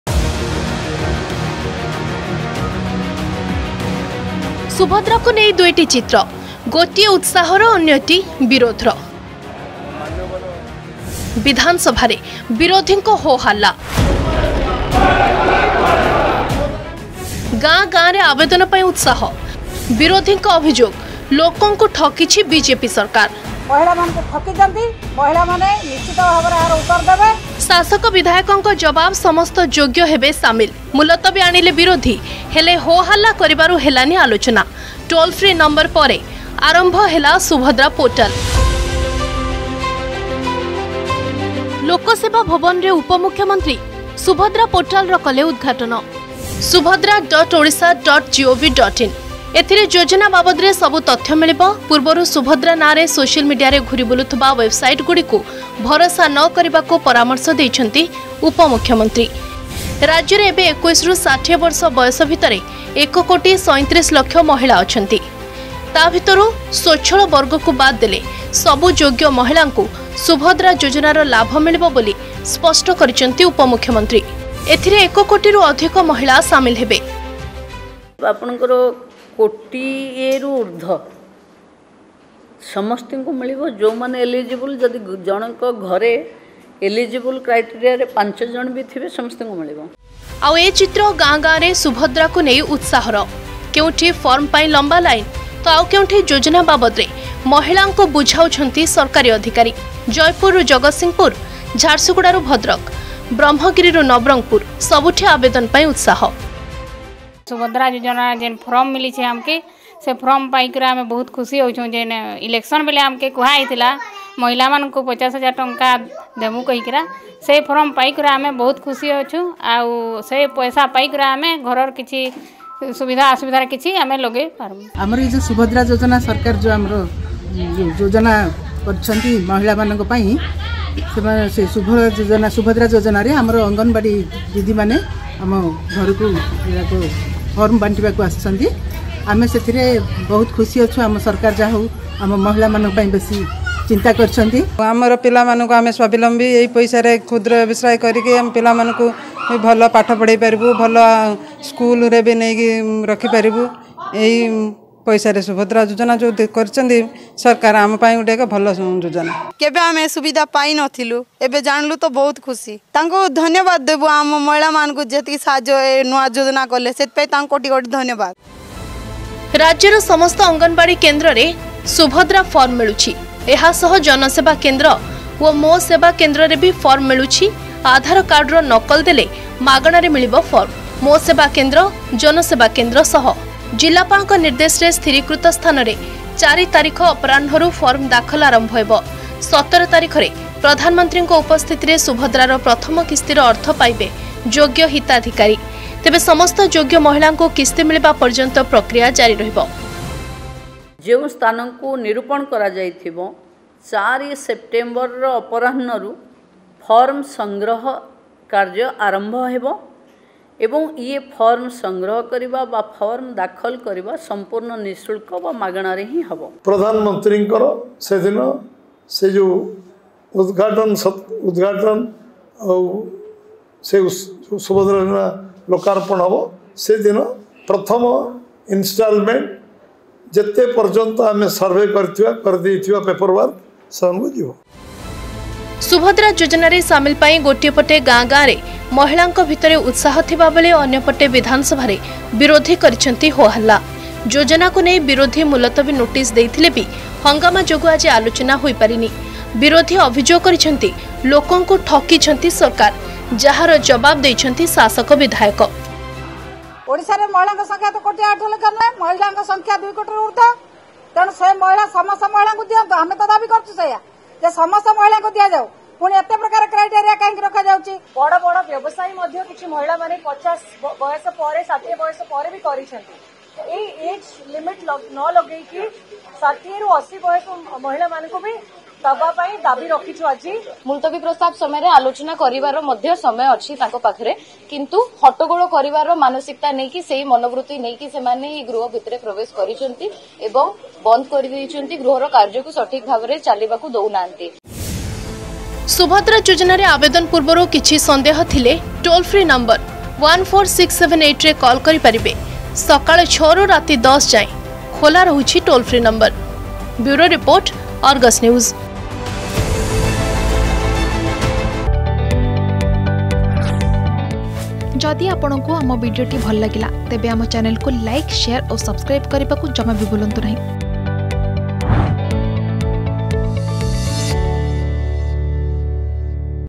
ସୁଭଦ୍ରାକୁ ନେଇ, ବିଧାନସଭାରେ ବିରୋଧୀଙ୍କ ହୋ ହା ଲ୍ଲା || Subhadra Yojana
ସୁଭଦ୍ରା ଯୋଜନାକୁ ନେଇ ଗାଁ ଗାଁରେ ଆବେଦନ ପାଇଁ  ବଢୁଛି ଉତ୍ସାହ । ସେପଟେ ବିଧାନସଭାରେ ବିରୋଧୀଙ୍କ ହୋ-ହାଲ୍ଲା । ବିରୋଧୀଙ୍କ ଅଭିଯୋଗ ହେଲା, ଲୋକଙ୍କୁ ଠକିଛି ବିଜେପି ସରକାର। ଏହାର ଜବାବ ଦେଇଛି ଶାସକ ଦଳ । ଶାସକଦଳ ବିଧାୟକ କହିଛନ୍ତି ସମସ୍ତ ଯୋଗ୍ୟ ହିତାଧିକାରୀ ଏହି ଯୋଜନାରେ ସାମିଲ ହେବେ ।